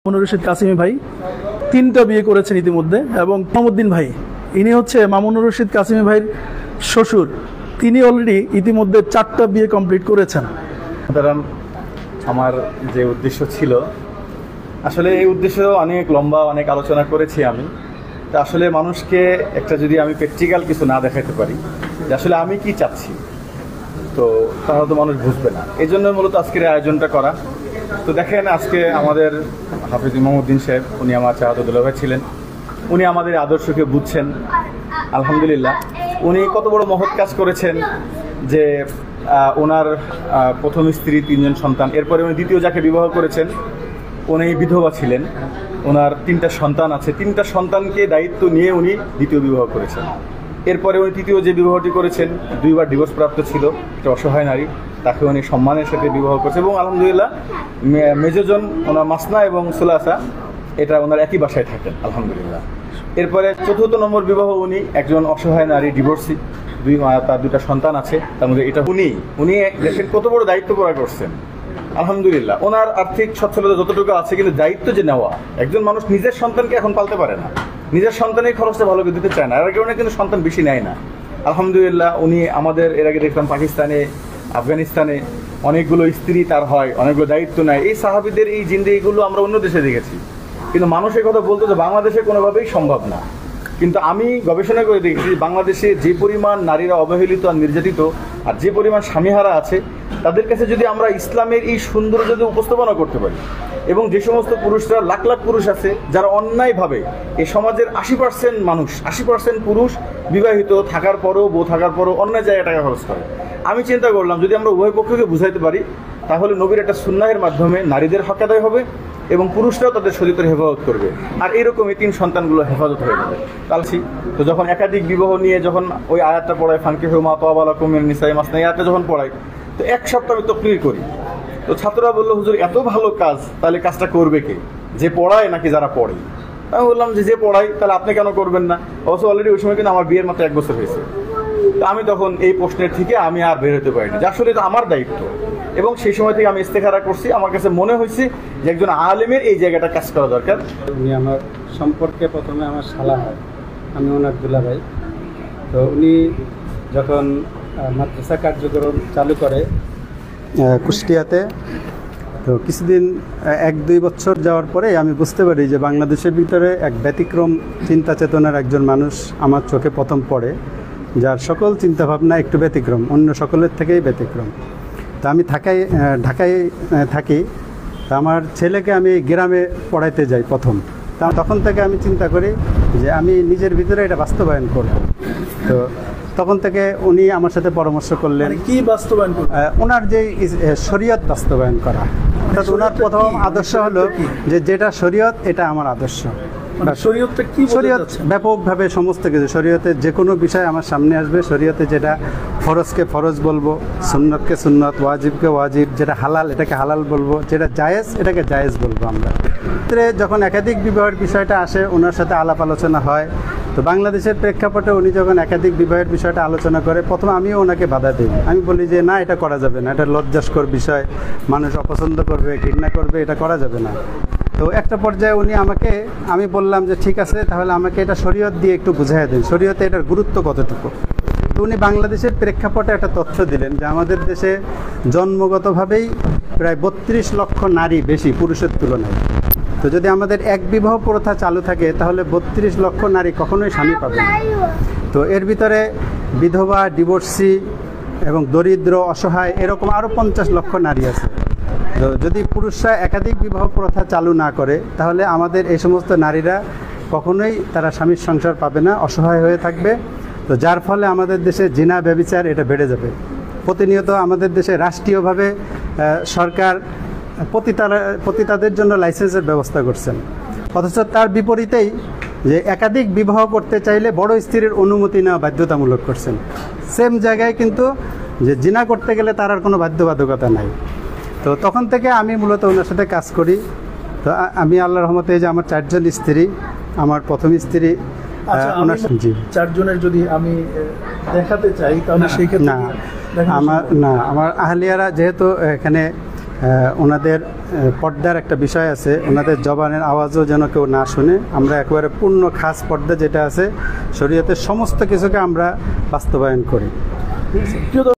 ১ বিয়ের চালু থাকলে ৩২ লক্ষ নারী কখনো স্বামী পাবে না,জামাই শ্বশুর করলেন ৭ বিয়ে | islamic Marriage
জামাই-শ্বশুর মিলে ইসলামী আর্দশের একাধিক বিয়ের প্রচার করছেন | islamic Marriage | marriage media